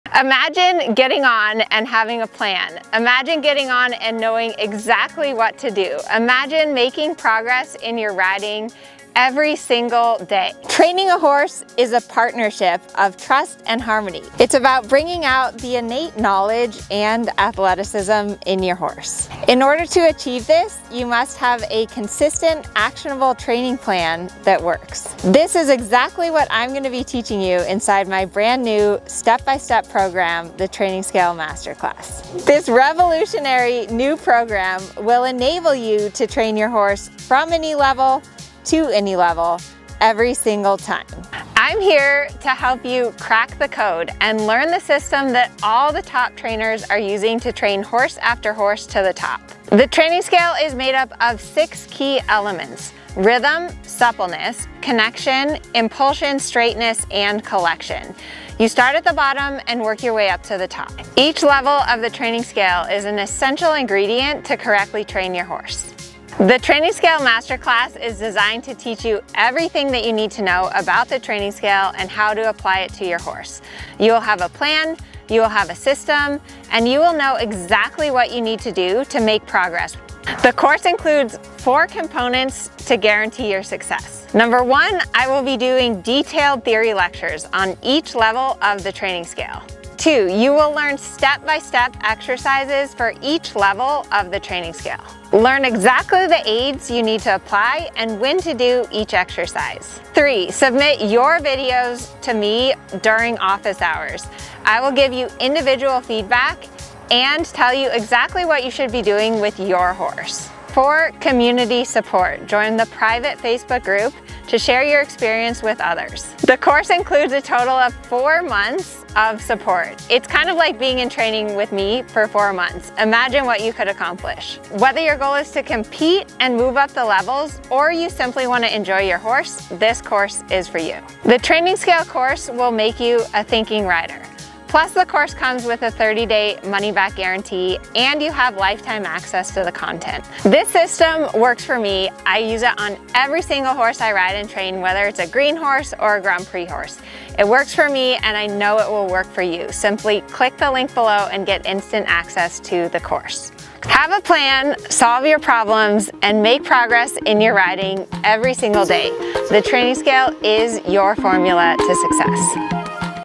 Introducing the Training Scale Masterclass
🥳Official Announcement!🥳Enrollment is now open for my Training Scale
Masterclass.

This is by far my favorite program I teach. Why? Because the Training Scale is so fundamental and essential to training horses. If you want to learn how you can make progress every single day- you need an actionable training plan that works. The Training Scale is your answer!

It's simple, it's effective, and it works for every horse, in every discipline. Now is your chance to learn the system that ALL the top riders use to train horse after horse up the levels.

I have hundreds of thousands of followers online who have used my techniques to achieve unparalleled levels of success in their riding. And if you’re willing to invest a little time now, you will get double the results in half the time. I guarantee it!

When you enroll in the Training Scale Masterclass, you will get:
1. The Training Scale Blueprint - theory lectures
2. Step - by step exercises - over 40+
3. Personalized coaching - submit your videos and get direct feedback
4. Community Support - in our private Facebook group

My programs transform riders like no other. You will have a plan, you will have a system, and you will be making progress in your riding every single day!

This is the only time this year I can offer this course (my calendar is full!), and seats are limited. Don't miss out on your chance to get in.